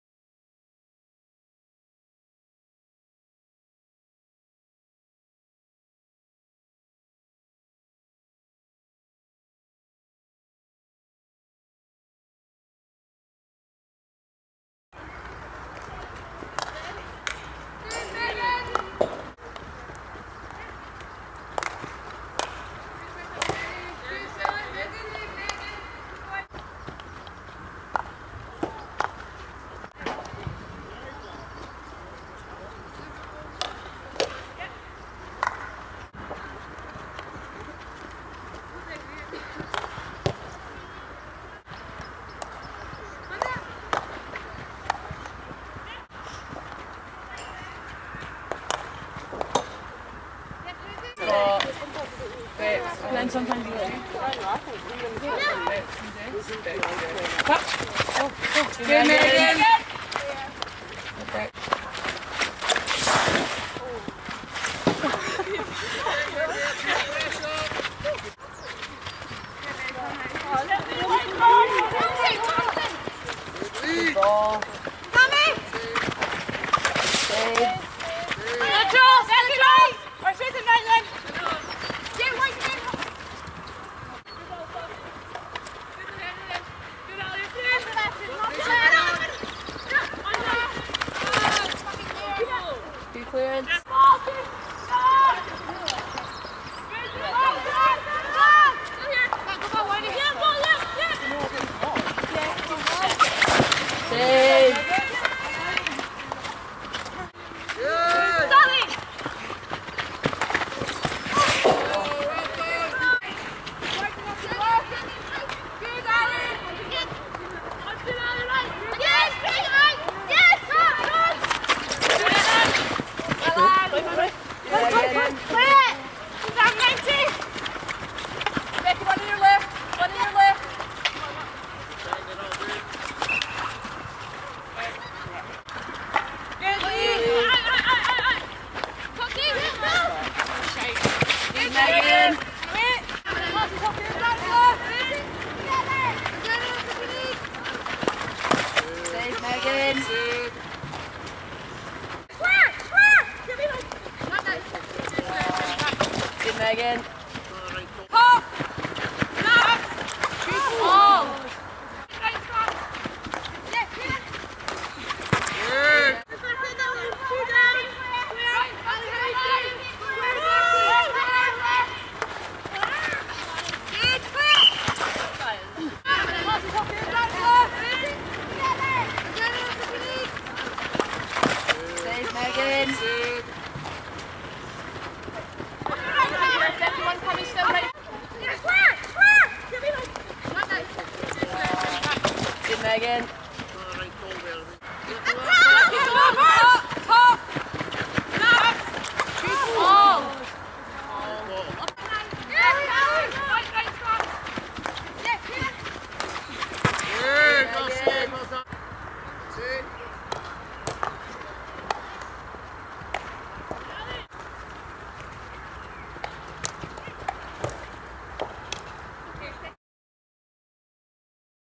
Megan Inch Scotland Senior Training
Stateside Sports Scholarships
Goalkeeper
Scotland U16s
Scotland U18s
Scotland U21s
Only member of U21s invited to train with Scotland Senior Women